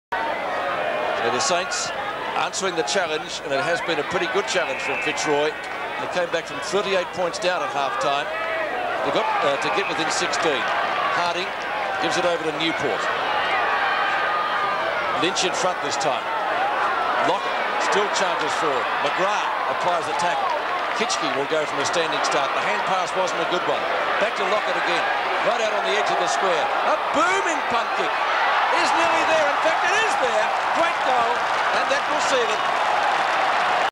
More Tony Lockett Goodness
16/06/91 - Tony pops though number 8 and the sealer